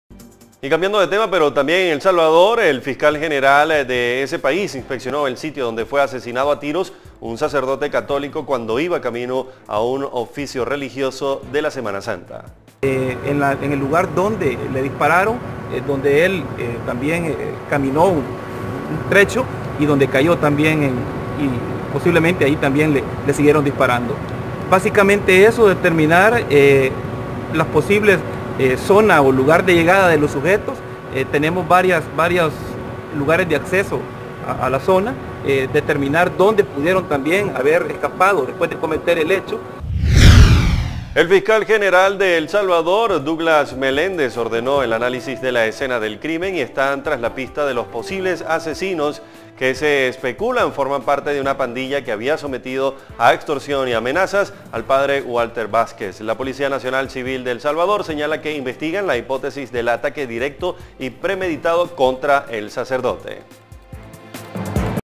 A sacerdote lo acribillaron dice fiscal general de El Salvador
En El Salvador el fiscal general inspeccionó el sitio donde fue asesinado a tiros un sacerdote católico cuando iba camino a un oficio de la semana santa.

El fiscal de El Salvador, Douglas Meléndez, ordenó el análisis de la escena del crimen y esta tras la pista de los posibles asesinos, que se especula forman parte de una pandilla, que había sometido a extorsión y amenazas al padre Walter Vásquez.

La Policía Nacional Civil señala que investigan la hipótesis del ataque directo y premeditado contra el sacerdote.